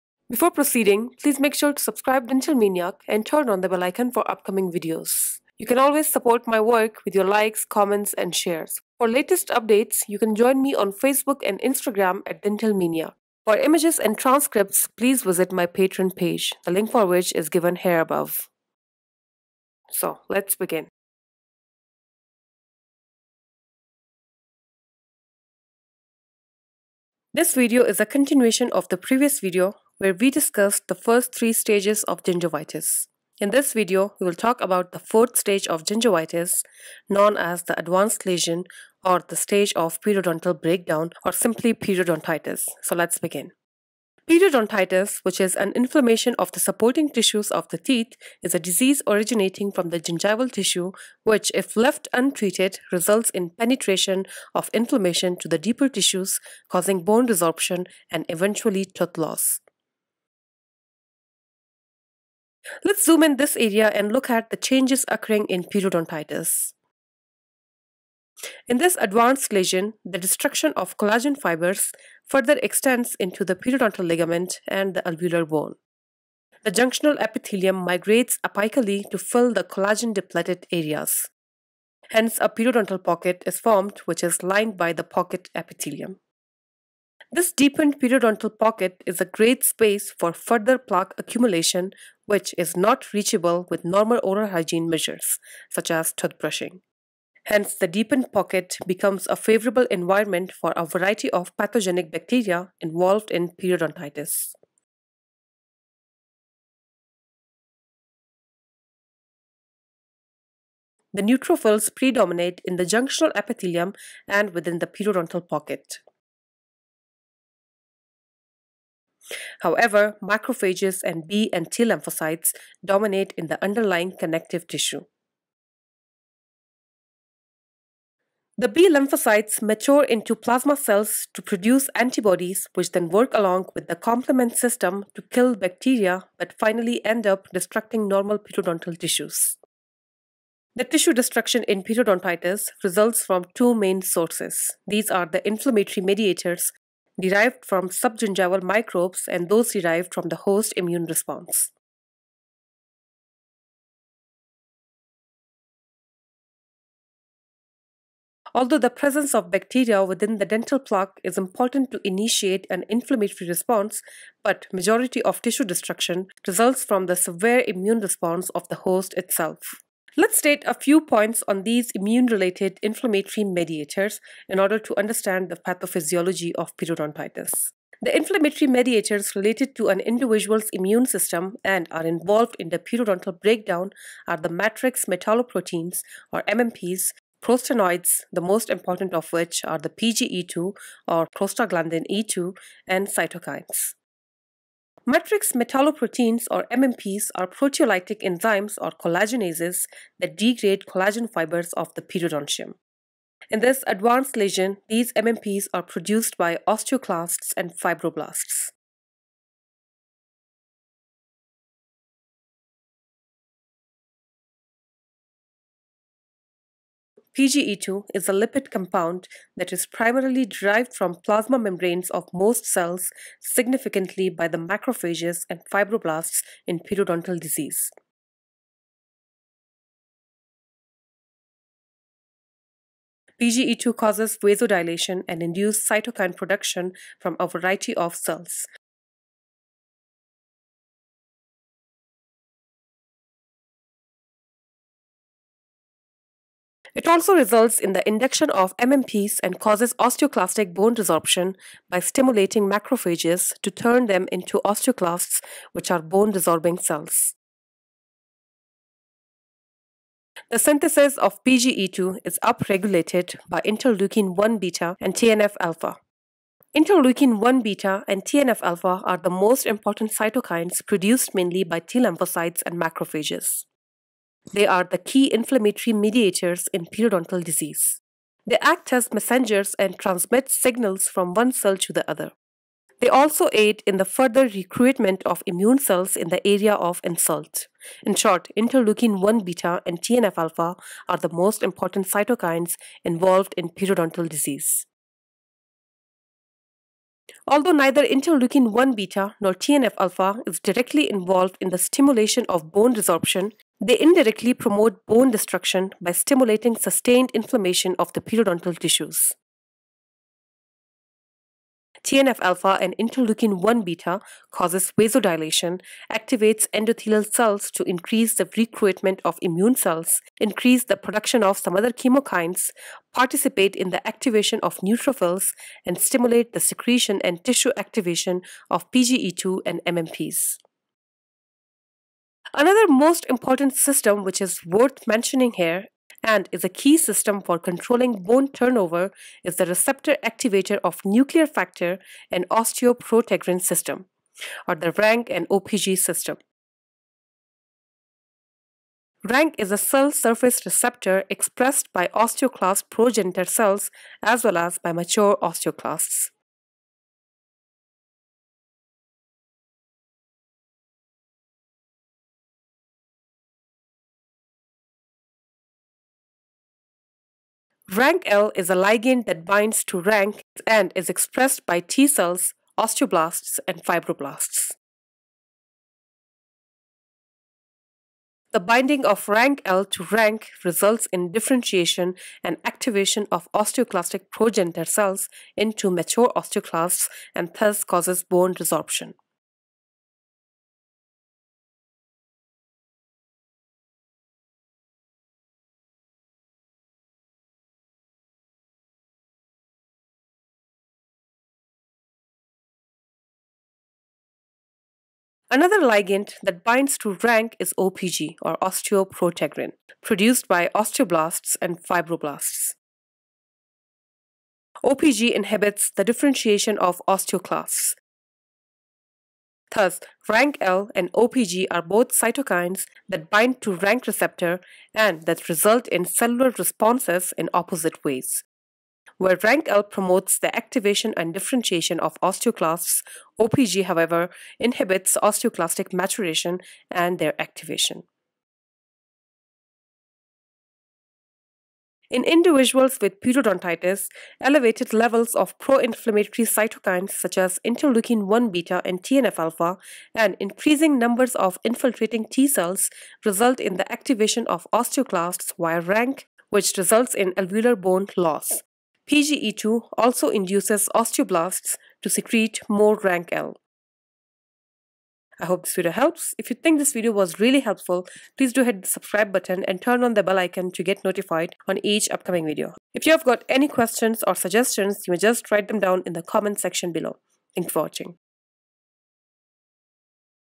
Periodontal Disease | Pathogenesis
This video is a continuation of the previous video where we discussed the first three stages of gingivitis. In this video, we will talk about the fourth stage of gingivitis known as the advanced lesion or the stage of periodontal breakdown or simply periodontitis. So let’s begin!
Periodontitis, which is an inflammation of the supporting tissues of the teeth is a disease originating from the gingival tissue which if left untreated results in penetration of inflammation to the deeper tissues causing bone resorption and eventually tooth loss.

Video (diagrams+editing) & Voiceover:
Dr.Maryam Kazman